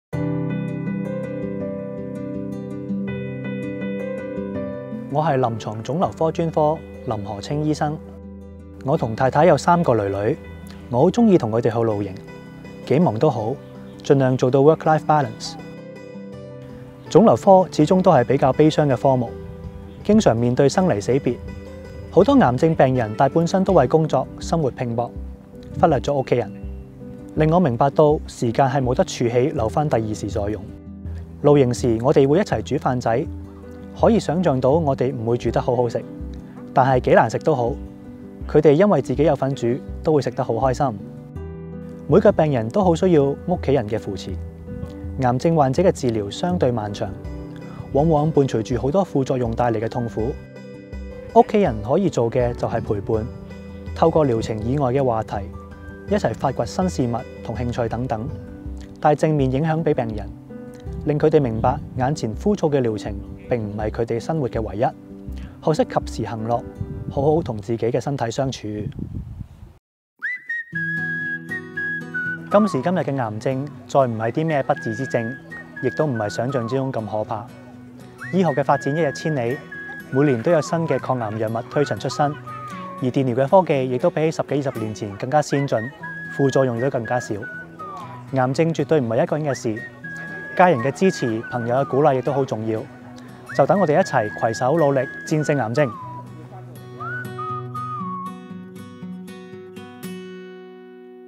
【白袍以外與你同行】Work Life Balance 享受家庭樂 ｜ 林河清醫生
就算工作幾忙都好，臨床腫瘤科專科林河清醫生都會同三個心肝寶貝去露營，盡量做到work life balance，因為家庭就係佢嘅精神支柱，工作嘅動力。

00:00 【白袍以外與你同行】Work Life Balance 享受家庭樂  ｜ 林河清醫生
00:11 林河清醫生喜歡和家人一起露營
01:10 林河清醫生給病人的話